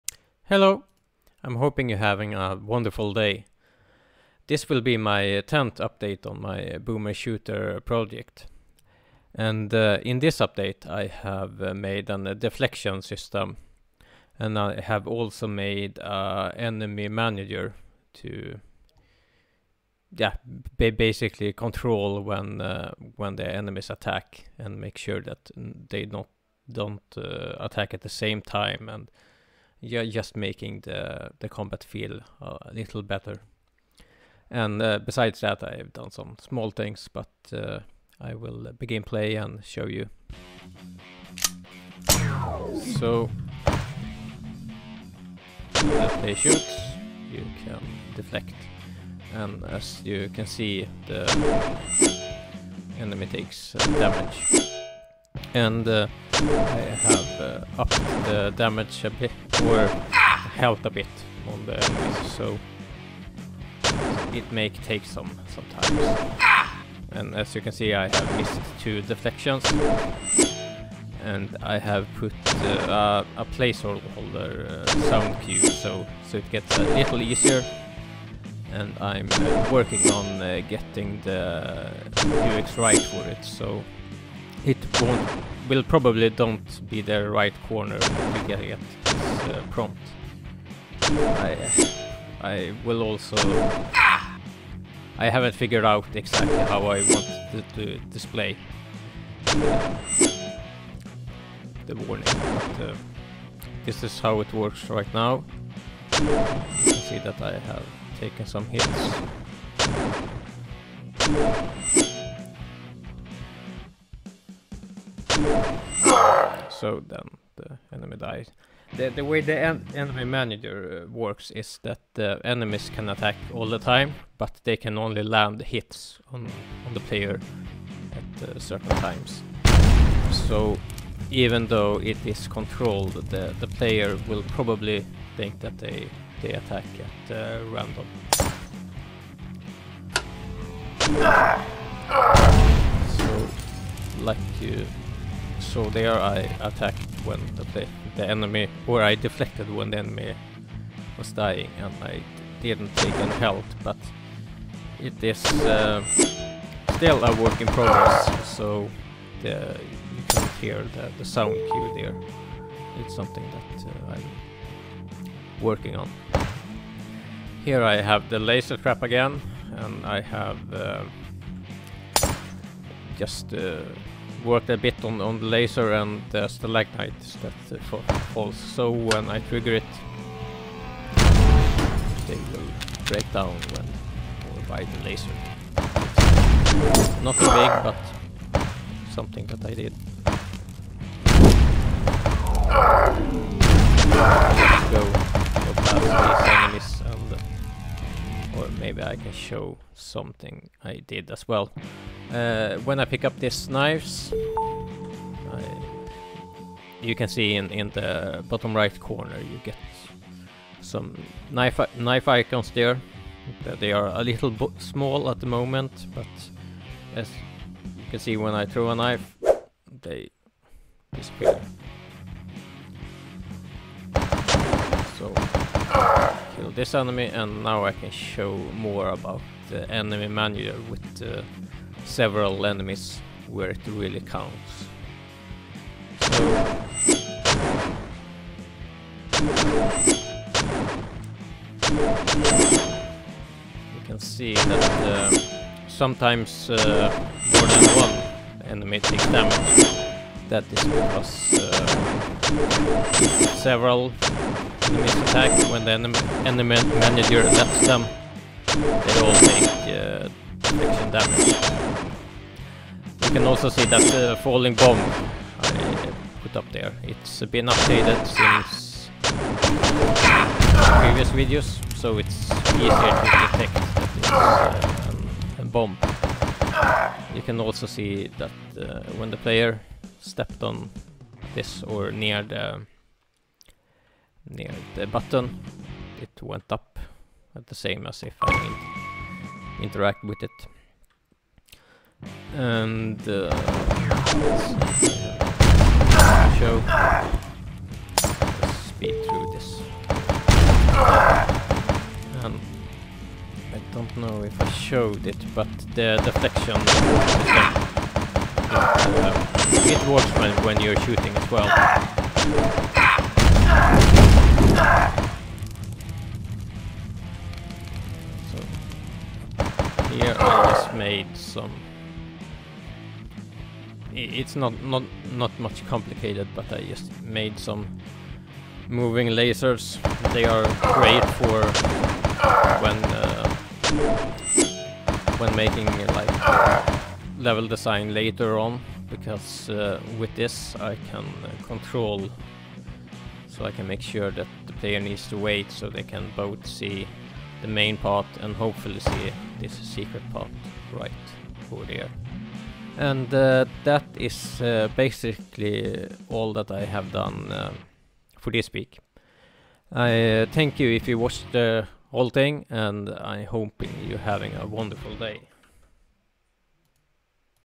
Boomer Shooter Update 10
In this update I have added a deflection mechanic and a enemy manager.